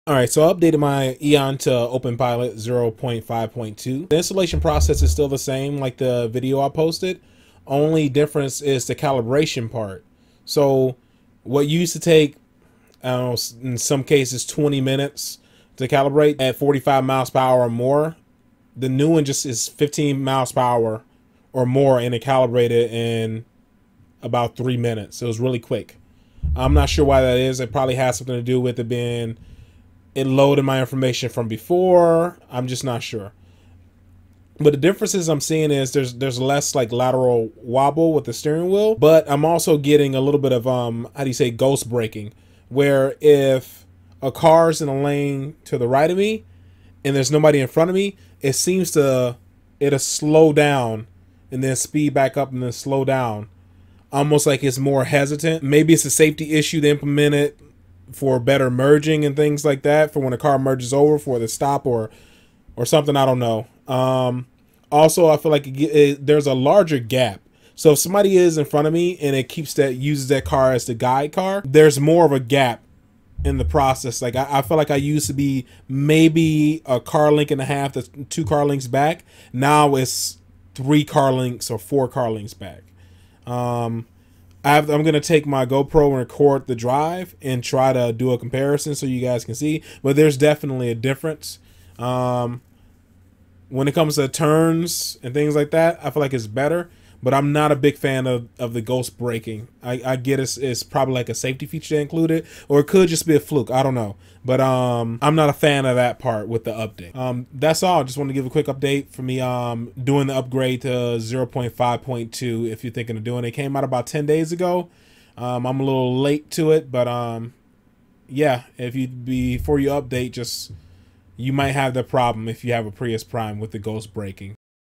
Eon Software Update Open Pilot 0.5.2 Thoughts (Prius Prime 2018 Premium edition )
Don't Click this Just an update on Comma Ai's Eon Software Update Open Pilot 0.5.2 (Prius Prime 2018 Premium edition )